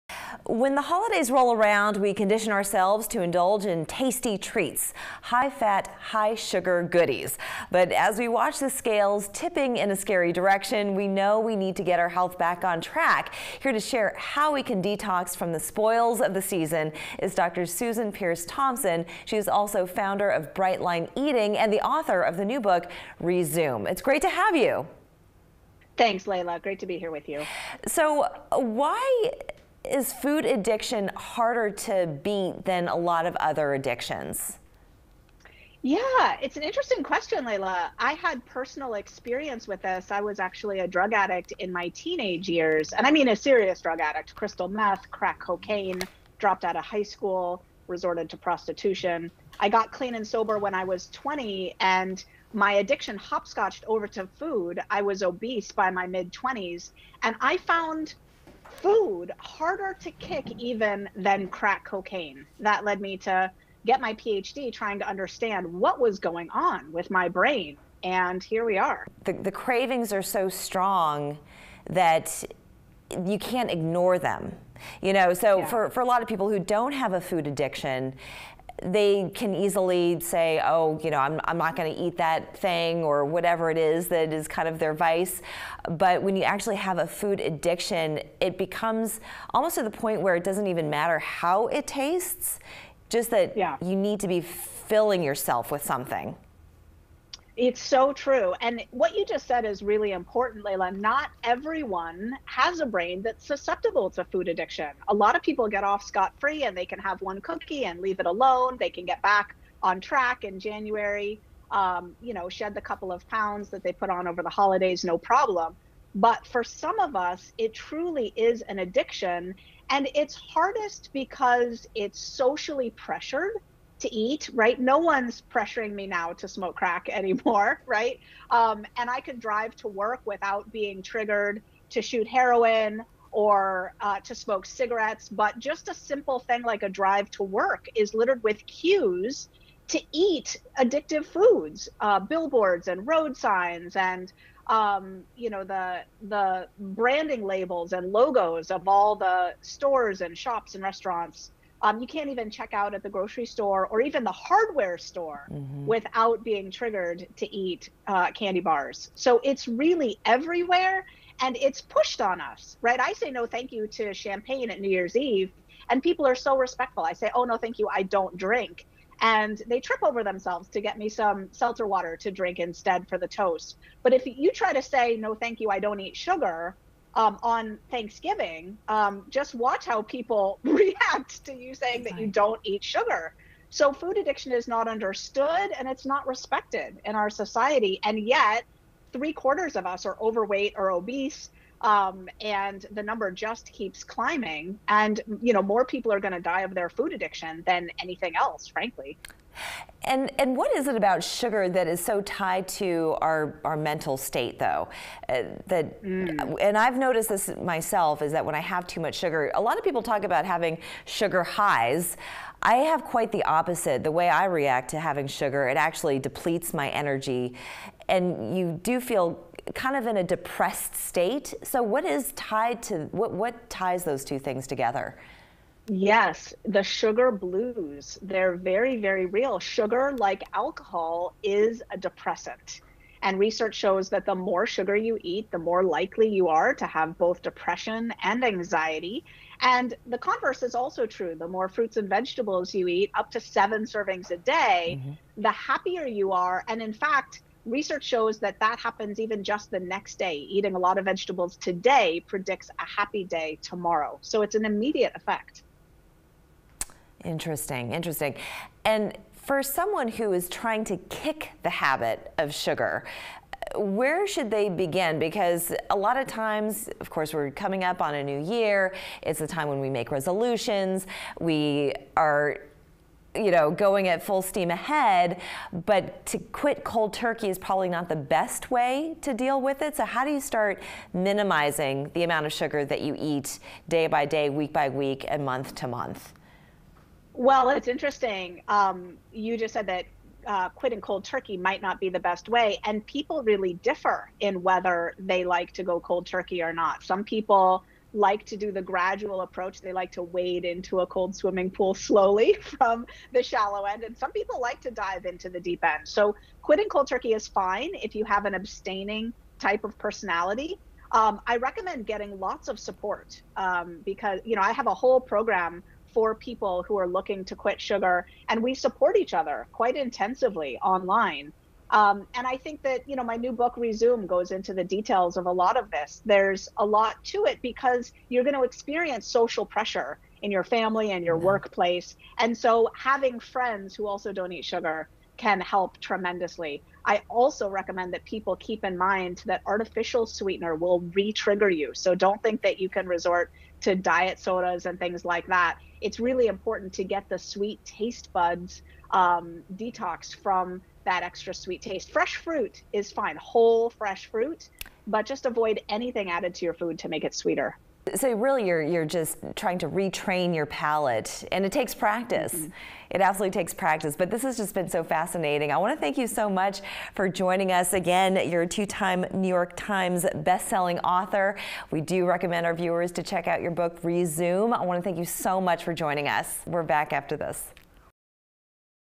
Changing Our Food Habits in the New Year | FOX 24 News Now
As many begin the journey of changing their food habits, a renowned addiction psychologist and best-selling author explains how to detox from sugar.